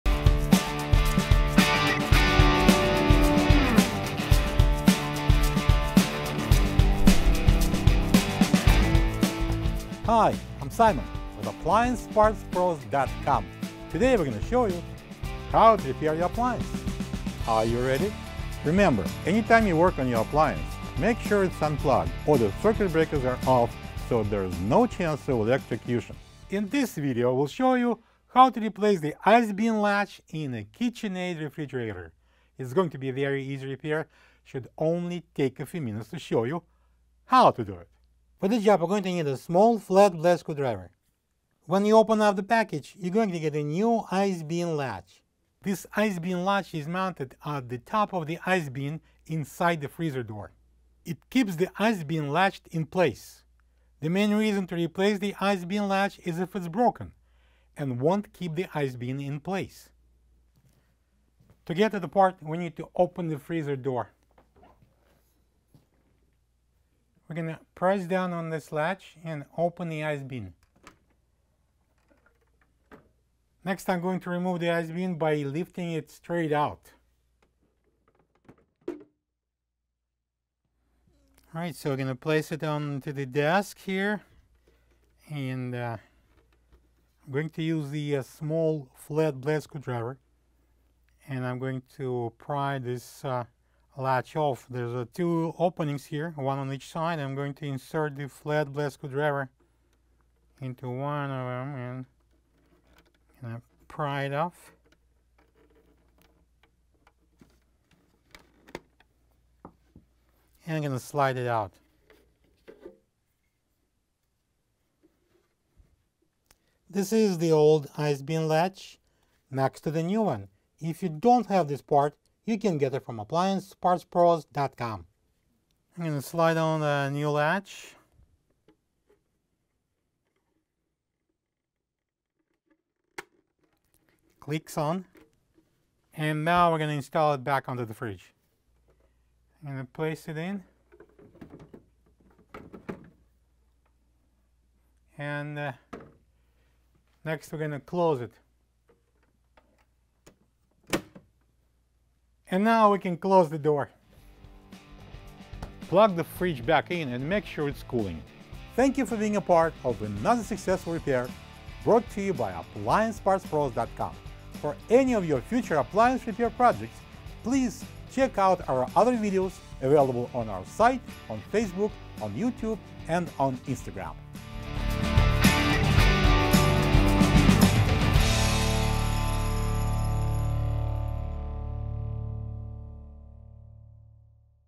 How To Replace: Whirlpool/KitchenAid/Maytag Refrigerator Ice Bin Latch W11652042
Symptoms: Broken and won't keep the ice bin in place.

Tools: Small flathead screwdriver.

This Whirlpool/KitchenAid/MaytagRefrigerator Ice Bin Latch replaces the following older part numbers on Whirlpool, Maytag, KitchenAid, Jenn-Air, Amana, Magic Chef, Admiral, Norge, Roper, Sears, Kenmore Refrigerators: WPW10258552, W10693542, W10258552.

0:00 Introduction
1:00 Removal of ice bin
1:21 Removal of old latch
2:05 Installing new latch/reinstalling ice bin
2:33 Final steps/closing remarks